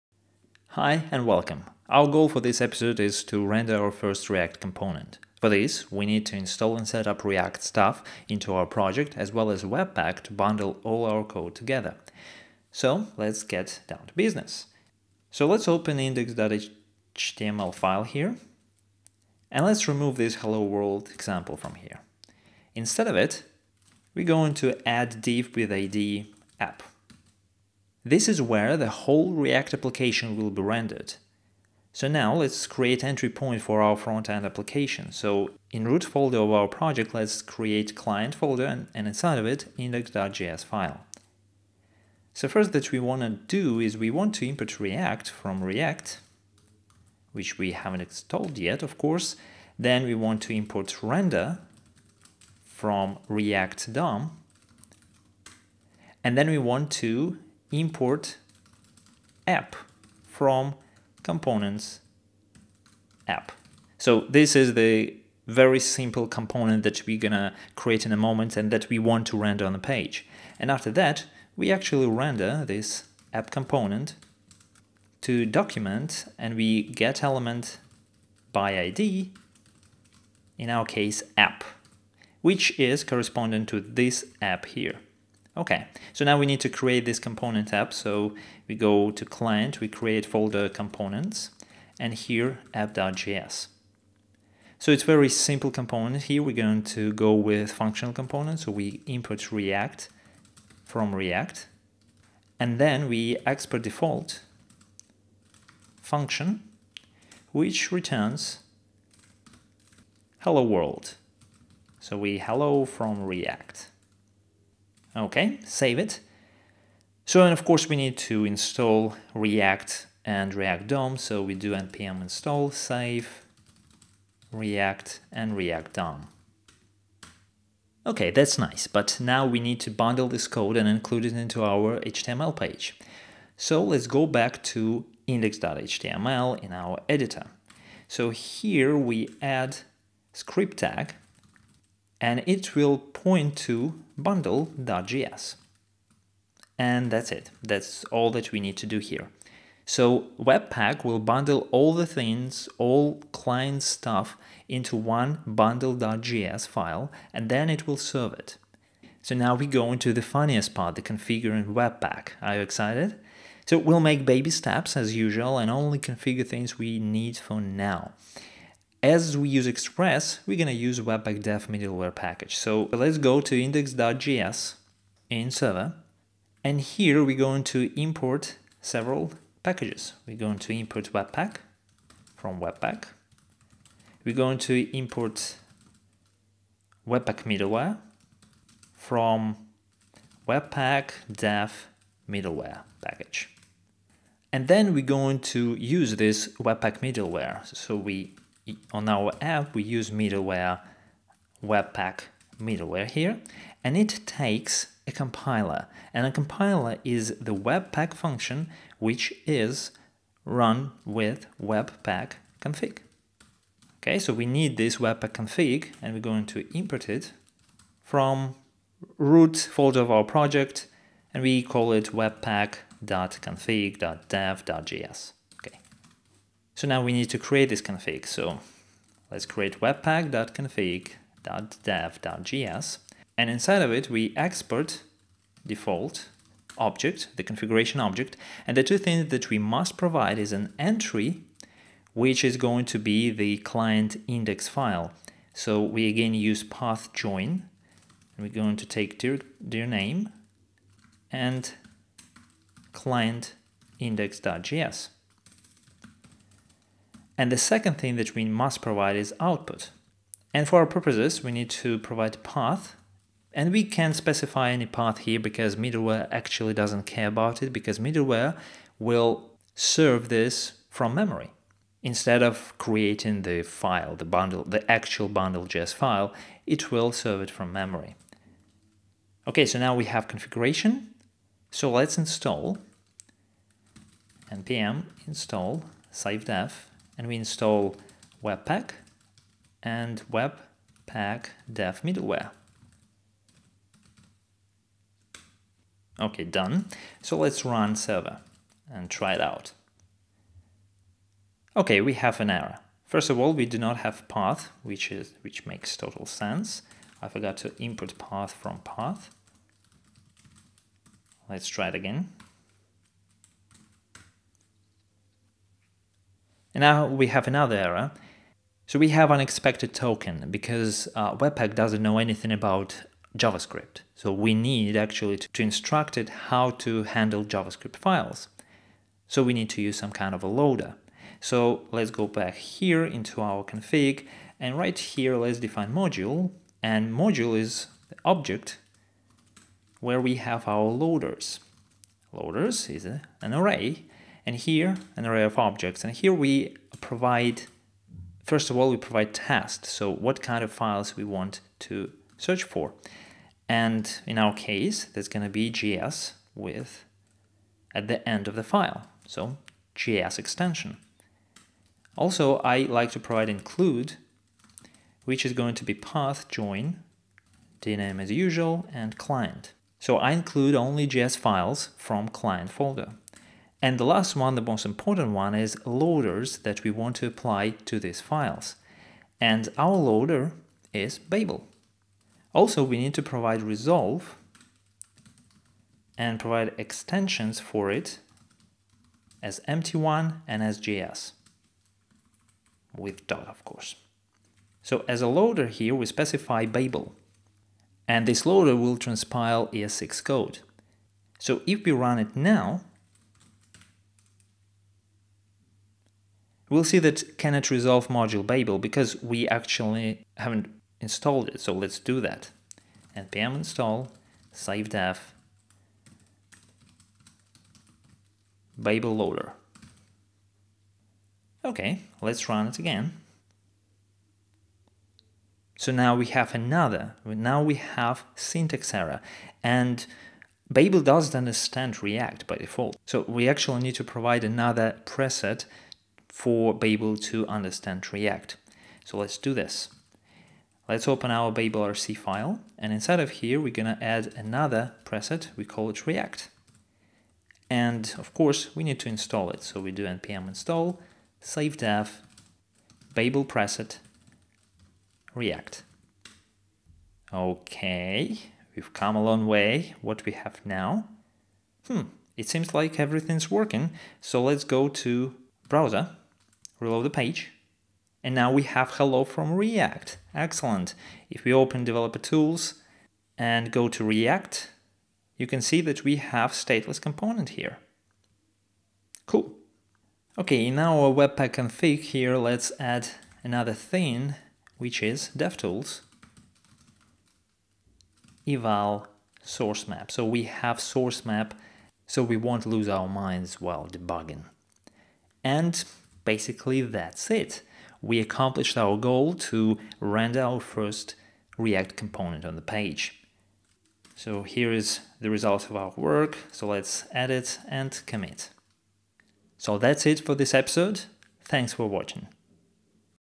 React & Redux #02. Project Setup: Render react component
The goal for this episode is to render our first react component. For this we need install and setup react stuff into our project as well as webpack to bundle all our code together.